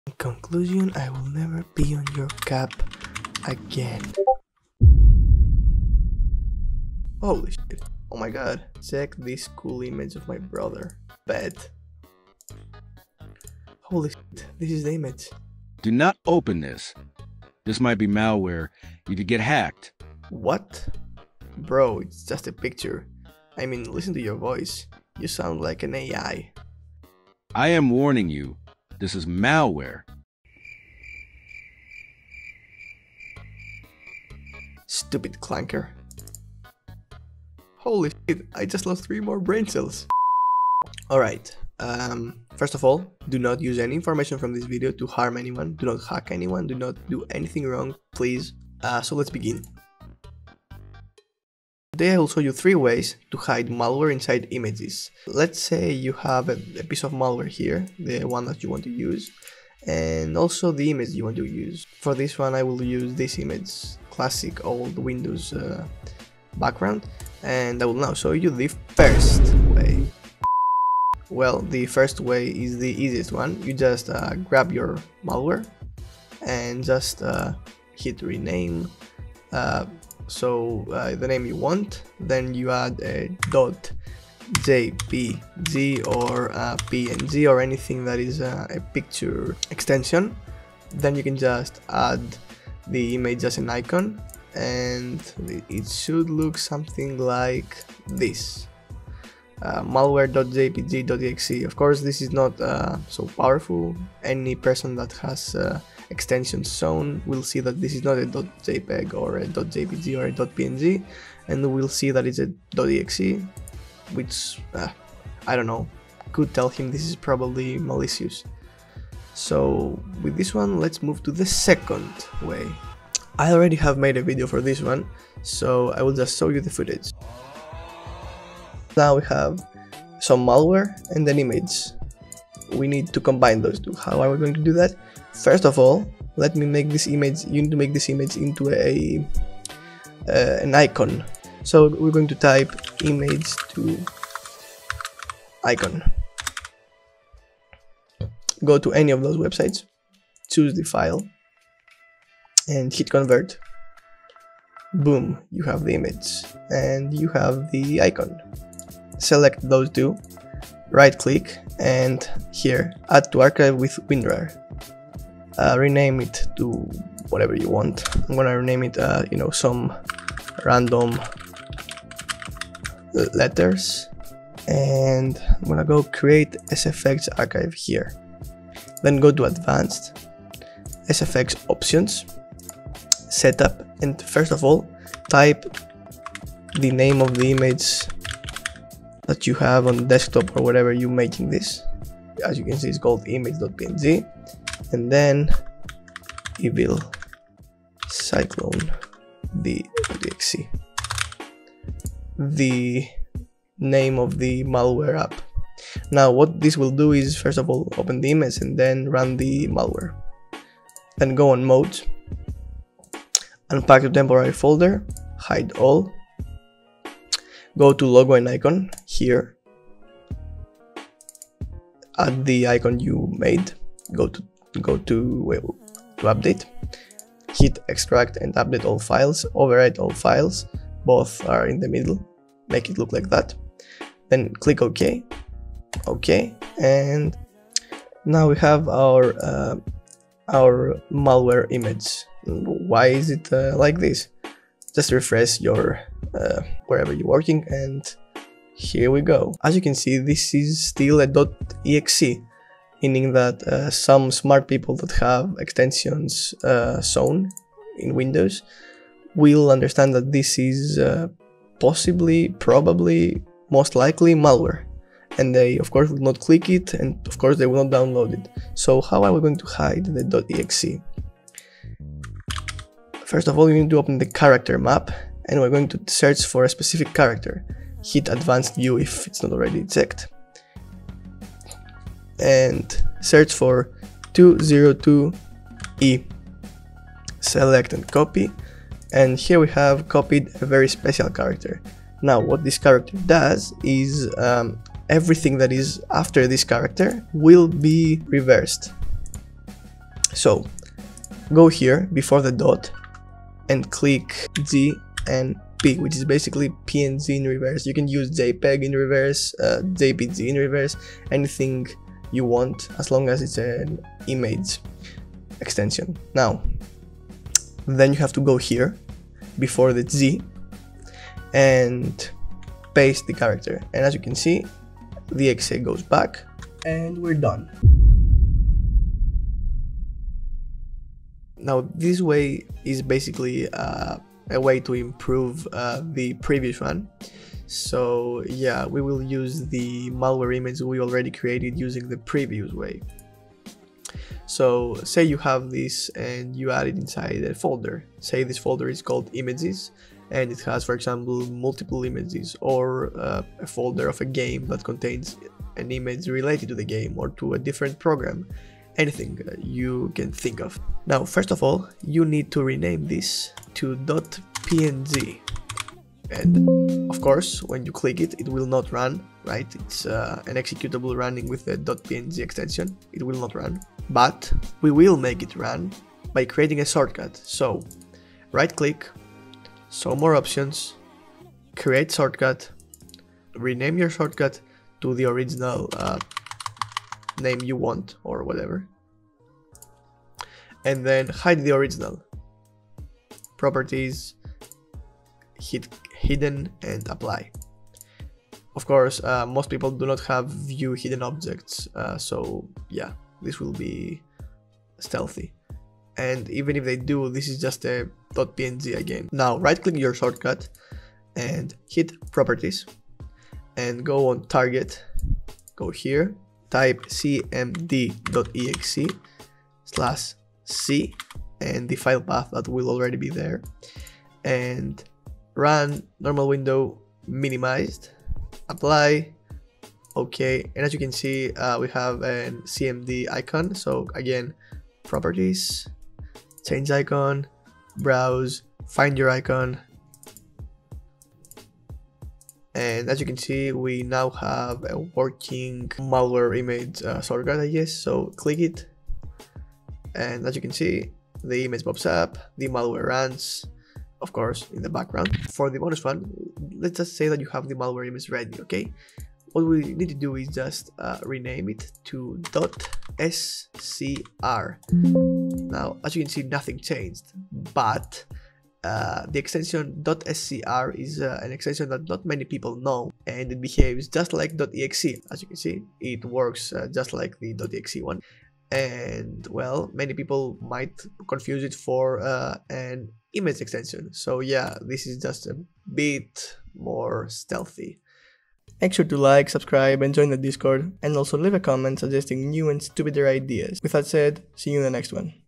How This Image Can Hack Your Computer
Have you ever wondered if a simple picture could hide a virus? 👀
In this video, I’m diving into one of the craziest tricks hackers use — hiding malware inside images. It looks harmless, but behind the pixels could be a dangerous payload ready to infect your system.

I’ll show you how this works, why it’s a big deal in cybersecurity, and what you can do to protect yourself. Whether you’re into hacking, tech, or just curious about how the dark side of the internet works, this is something you don’t want to miss.

💻 Watch till the end if you want to understand how attackers hide malware in plain sight and how to stay one step ahead.